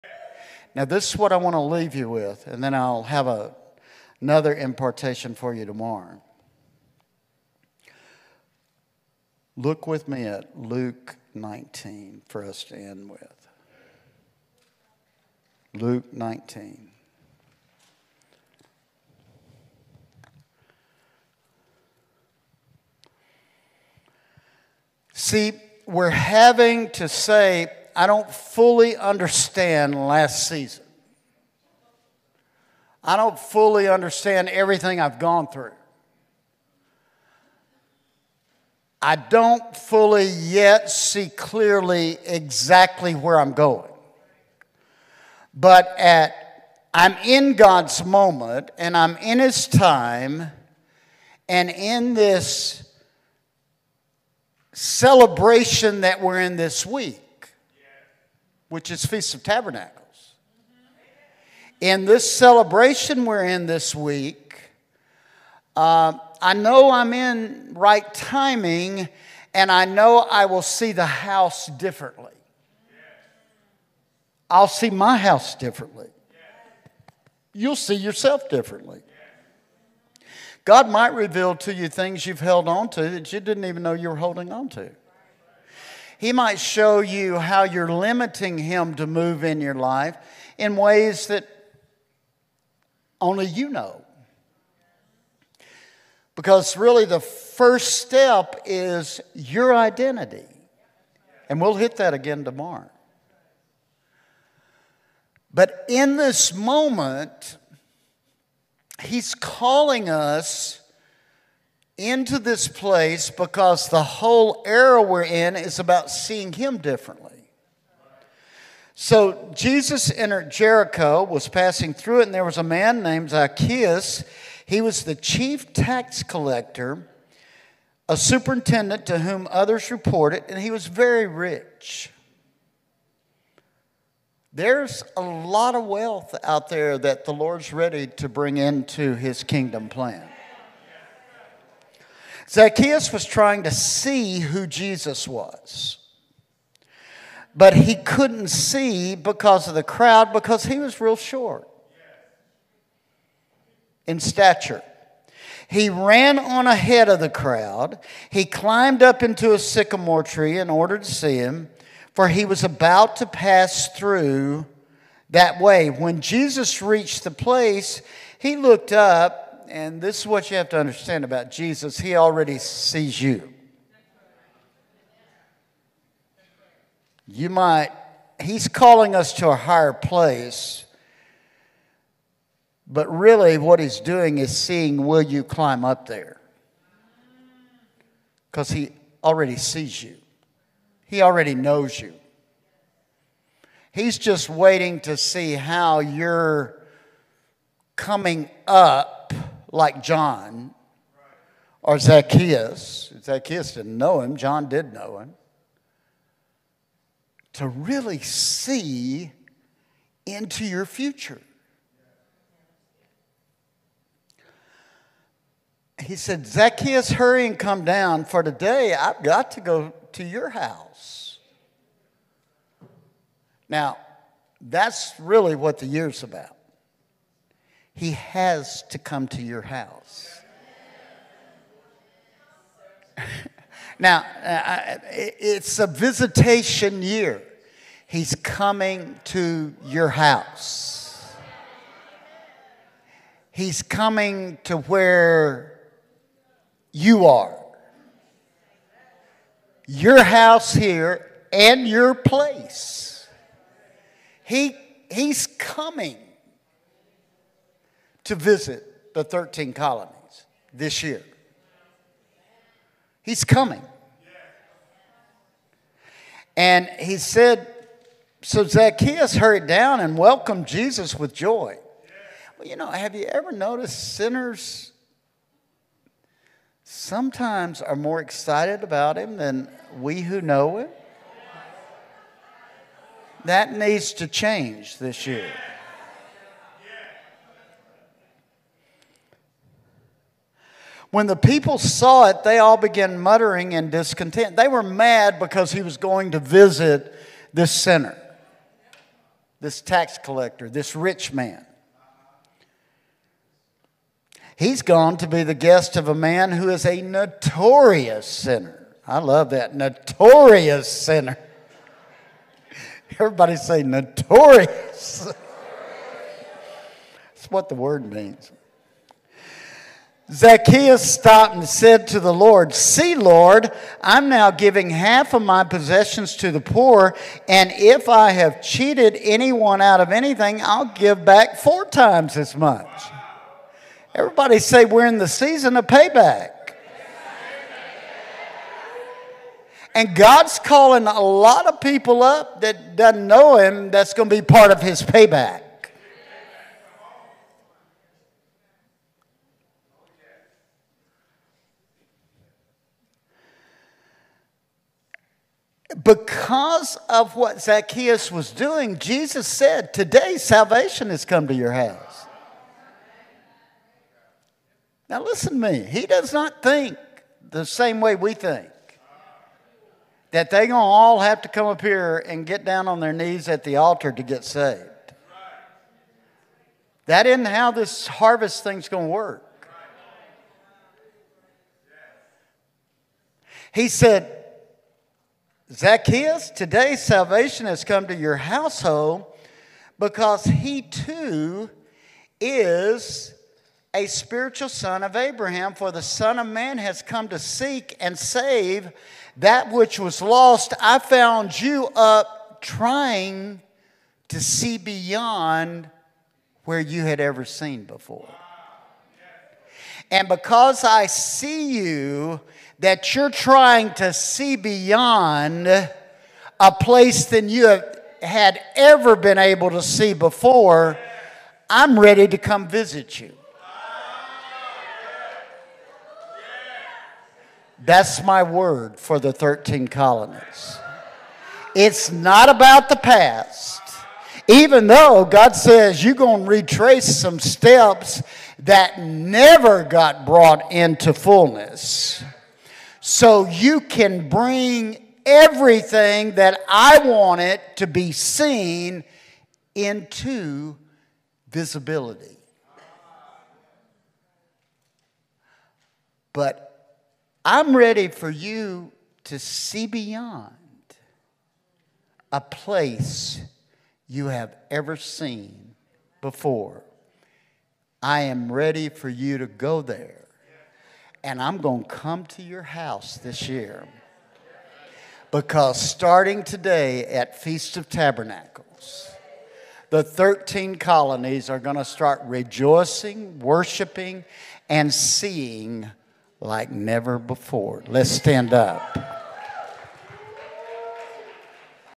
Chuck Pierce 2021 Prophetic Word: 5782 is the Year of the Lord’s Visitation (Luke 19:5)
Chuck speaks at King of Kings Worship Center in Basking Ridge, NJ, on 9/25/2021. Quotes: “This year He's calling us to a higher place but really what he's doing is seeing will you climb up there because he already sees you. He already knows you. He's just waiting to see how you're coming up. Luke 19:5 "He said, "Zacchaeus, hurry and come down for today I've got to go to your house." Now, that's really what the year's about. He has to come to your house - it's a visitation year. He's coming to your house. He's coming to where you are... your house here and your place.”

Be blessed.

This year we are having to say, "I don't fully understand last season. I don't fully understand everything I've gone through. I don't fully yet see clearly exactly where I'm going, but I'm in God's moment and I'm in his time." And in this celebration that we're in this week, which is feast of Tabernacles. "In this celebration we're in this week, I know I'm in right timing. I know I will see the house differently. I'll see MY house differently." You'll see yourself differently.

God might reveal to you things you've held onto that you didn't even know you're holding on to. He might show you how you're limiting him to move in your life in ways that only you know. Because really the first step is your identity. And we'll hit that again tomorrow. But in this moment, he's calling us into this place because the whole era we're in is about seeing him differently.

Luke 19:1 "So, Jesus entered Jericho, was passing through it, and there was a man named Zacchaeus. He was the chief tax collector, a superintendent to whom others reported, and he was very rich." There's a lot of wealth out there that the Lord is ready to bring into his kingdom plan.

Luke 19:3 "Zacchaeus was trying to see who Jesus was but he couldn't see because of the crowd.
because he was real short in stature. He ran on ahead of the crowd. He climbed up into a sycamore tree in order to see him for he was about to pass through that way. When Jesus reached the place, he looked up..." and this is what you have to understand about Jesus: he already sees you.

“This year He's calling us to a higher place but really what he's doing is seeing will you climb up there because he already sees you. He already knows you. He's just waiting to see how you're coming up. Luke 19:5 "He said, "Zacchaeus, hurry and come down for today I've got to go to your house." Now, that's really what the year's about. He has to come to your house - it's a visitation year. He's coming to your house. He's coming to where you are... your house here and your place.”

Luke 19:6 "So, Zacchaeus hurried down and welcomed Jesus with joy."
Well, you know, have you ever noticed sinners sometimes are more excited about him than we who know him? That needs to change this year.

Luke 19:7 "When the people saw it, they all began muttering in discontent."
They were mad because he was going to visit this sinner, this tax collector, this rich man.

Luke 19:7-8 "He's gone to be the guest of a man who is a notorious sinner. Zacchaeus stopped and said to the Lord, "See Lord, I'm now giving half of my possessions to the poor and if I have cheated anyone out of anything, I'll give back four times as much."

Everybody say, “We're in the season of payback." And God's calling a lot of people up that don't know him that's going to be part of his payback. Because of what Zacchaeus was doing, Jesus said, “Today, salvation has come to your house."

He does not think the same way we think... that they are going to all have to come up here to the altar and get down on their knees at the altar to get saved. That isn't how this harvest thing's going to work. He said, "Zacchaeus, today salvation has come to your household, because he too is a spiritual son of Abraham for the son of man has come to seek and save that which was lost." I found you up trying to see beyond where you had ever seen before. And because I see you that you're trying to see beyond a place that you had ever been able to see before, I'm ready to come visit you.

That's my word for the 13 colonies. It's not about the past, even though God says, "You're going to retrace some steps that never got brought into fullness so you can bring everything that I wanted to be seen into visibility. But I'm ready for you to see beyond a place you have ever seen before. I am ready for you to go there and I'm going to come to your house this year."